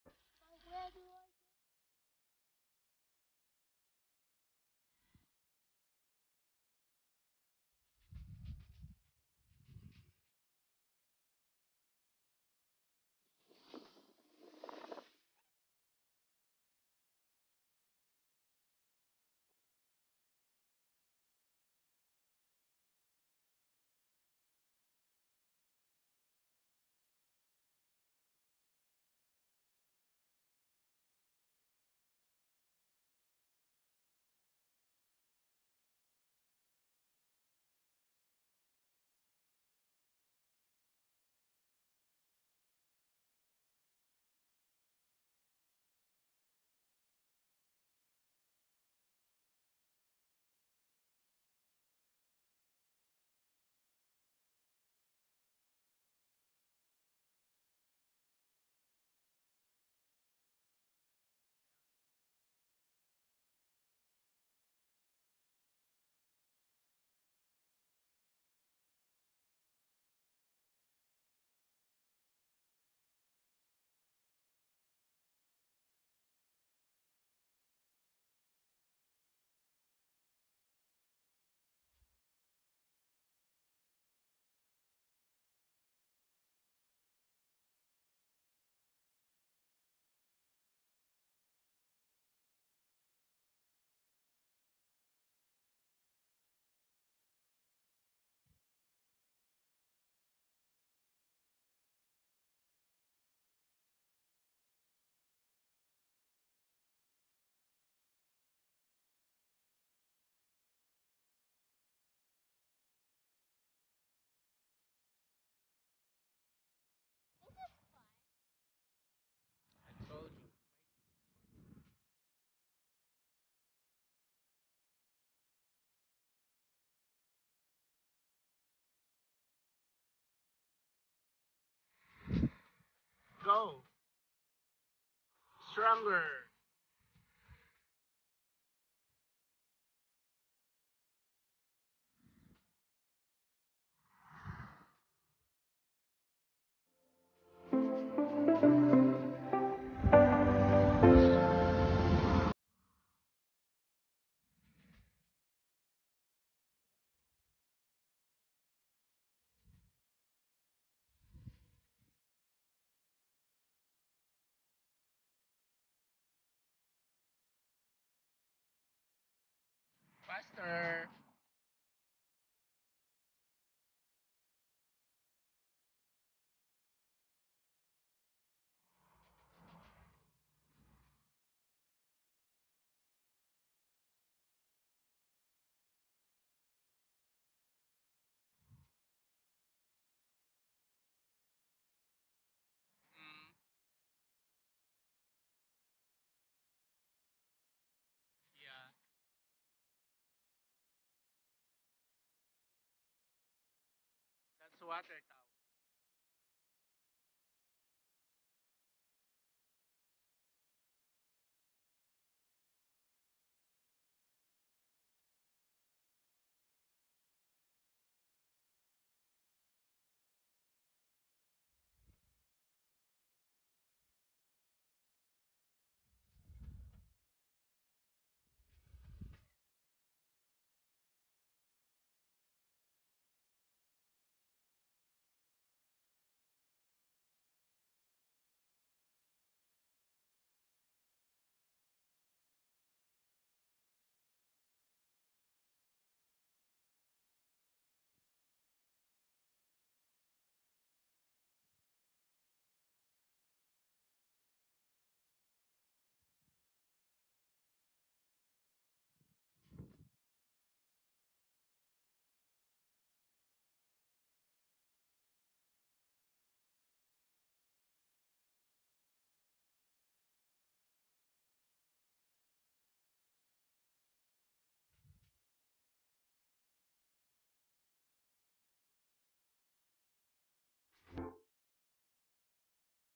Bike along Oulu Park
Biking along the riverbanks.
Its spring season but temperetures still dropping at -6 . Enjoy.